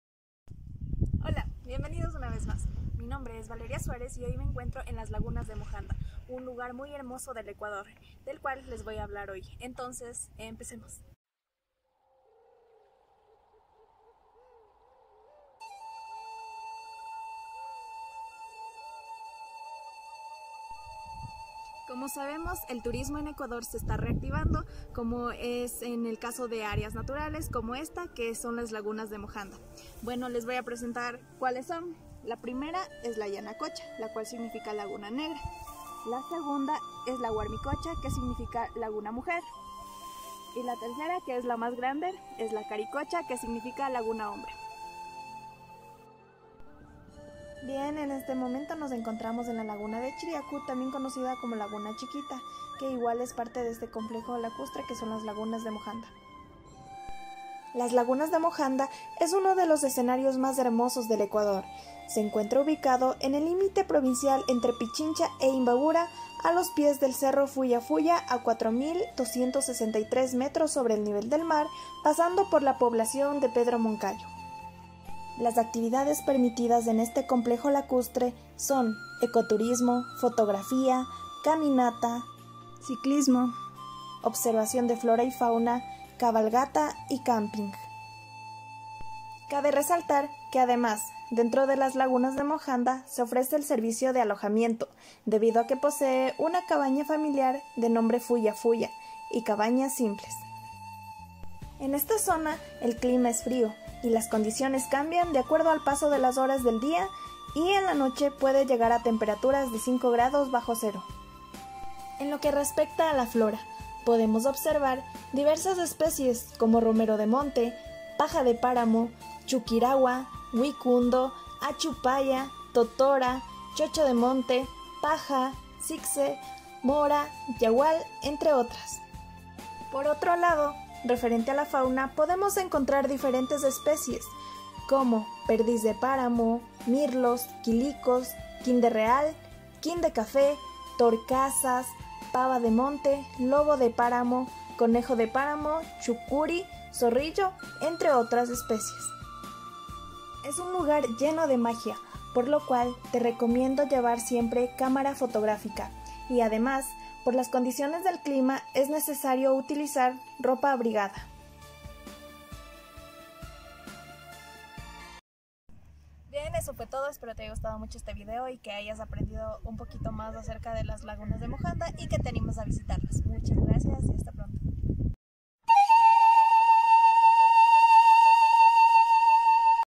Las Lagunas de Mojanda
El complejo lacustre lagunas de mojanda es uno de los lugares naturales que somos muy afortunados de poder visitar, por favor más conciencia, hay vida dentro, muchas especies que están en su hábitat natural su hogar, cuidemos estos sitios, NO MÁS INCENDIOS.